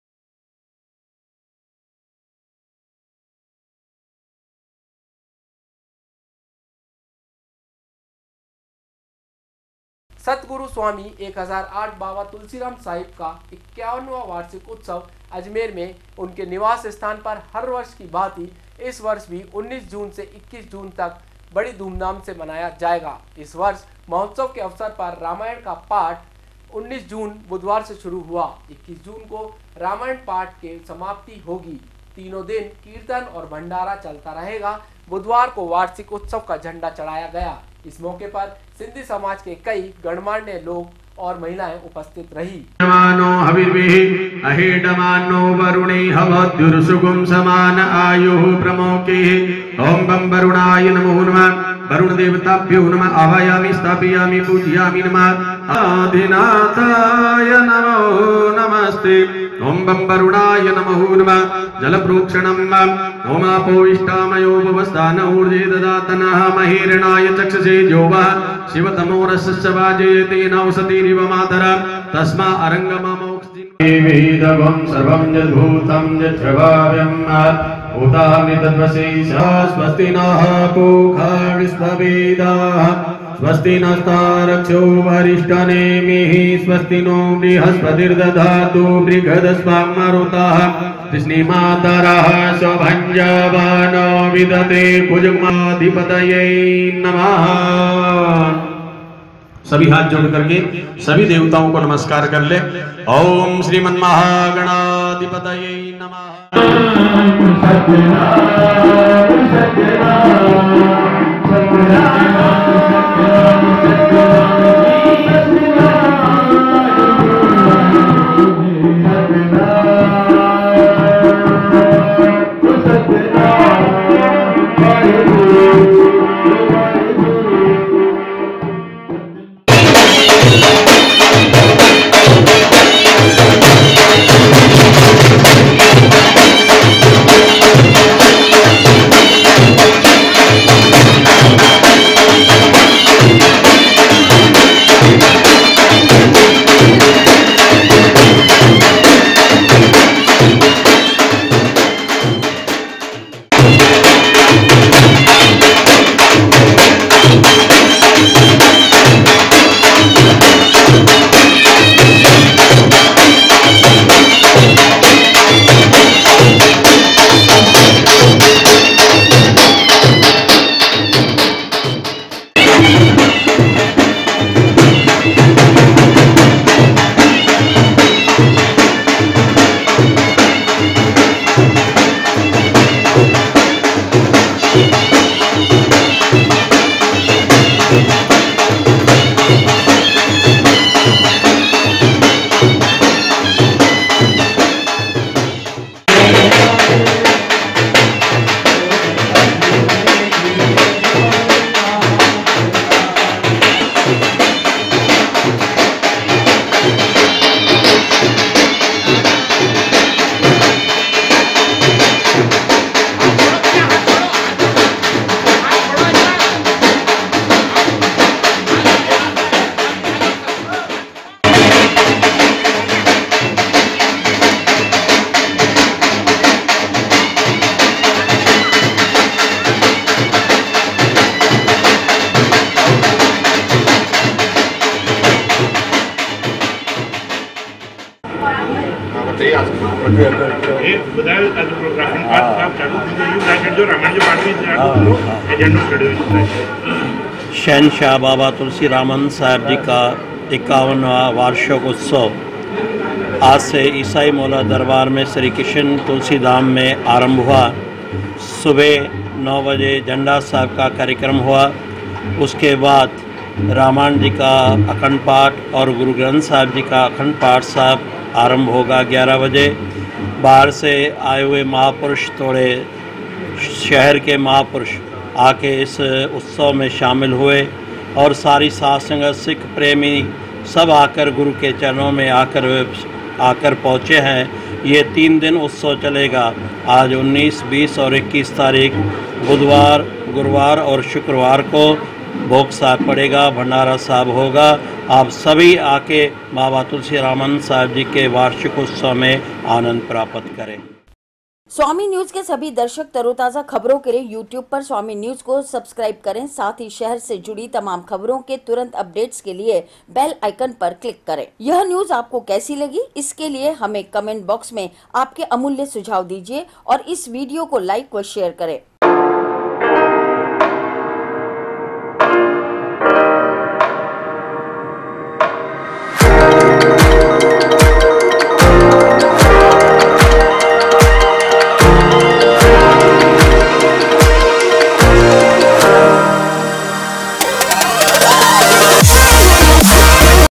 सतगुरु स्वामी 1008 बाबा तुलसीराम साहिब का 51 वां वार्षिकोत्सव झंडारोहण के साथ शुरू
19 June 2019,
    सतगुरु स्वामी 1008 बाबा तुलसीराम साहिब का 51 वार्षिक उत्सव अजमेर में उनके निवास स्थान पर हर वर्ष की भांति इस वर्ष भी 19 जून से 21 जून तक बड़ी धूमधाम से मनाया जाएगा इस वर्ष महोत्सव के अवसर पर रामायण का अखंड पाठ 19 जून बुधवार से शुरू हुआ 21 जून को रामायण के पाठ की समाप्ति होगी तीनों दिन कीर्तन और भंडारा चलता रहेगा बुधवार को वार्षिक उत्सव का झंडा चढ़ाया गया इस मौके पर सिंधी समाज के कई गणमान्य लोग उपस्थित रहे।.